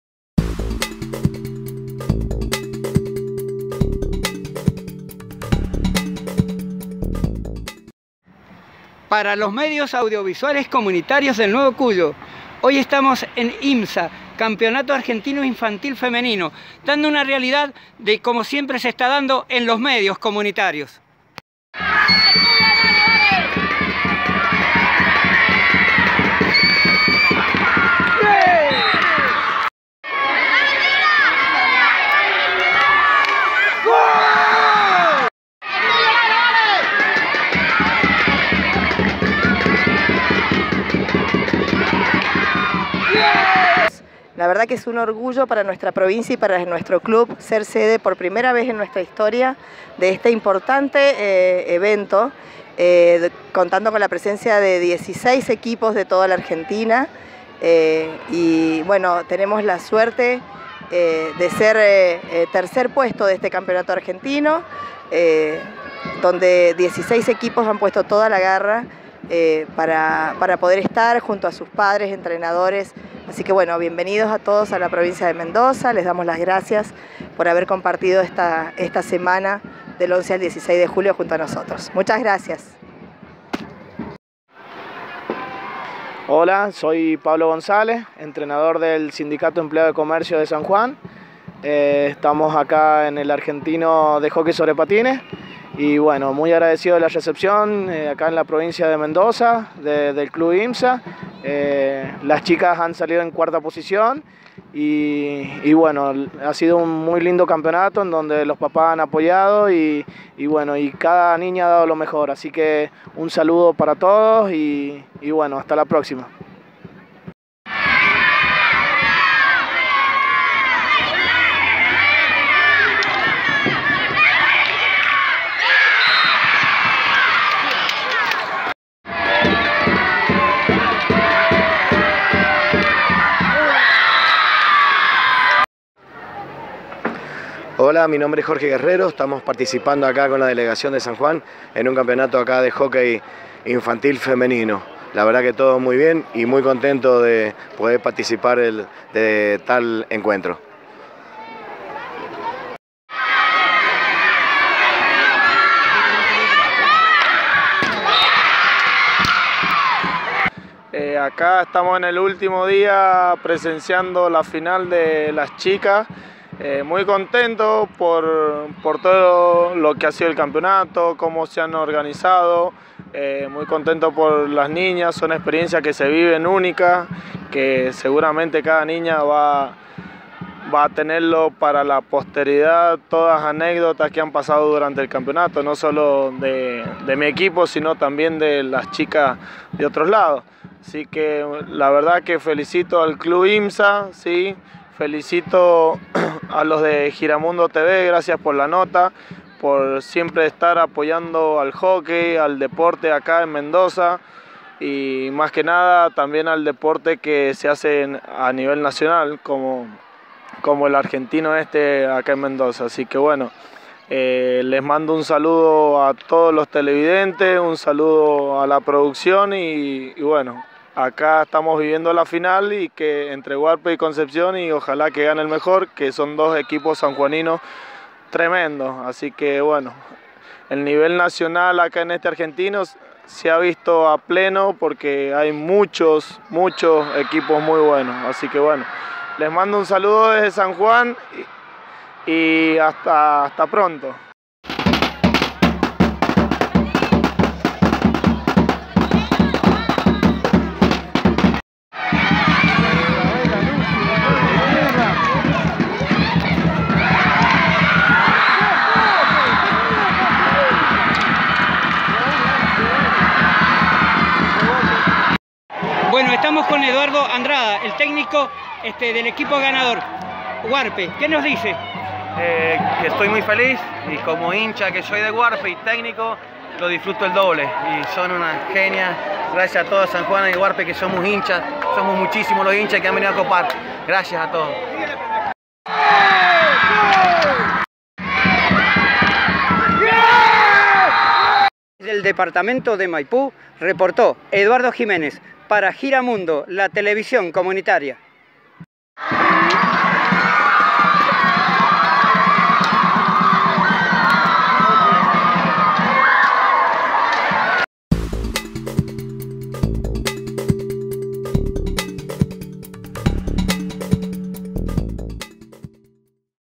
Campeonato argentino de hockey femenino en Mendoza
Entre el 11 al 16 de julio de 2022 se llevó a cabo el Campeonato Argentino de Hockey Infantil Femenino. Diciséis equipos del país se congregaron en el Club IMPSA para compartir días de encuentro y deporte. El corresponsal popular Eduardo Giménez participó de la última fecha en la que resultó ganador el equipo sanjuanino Huarpes.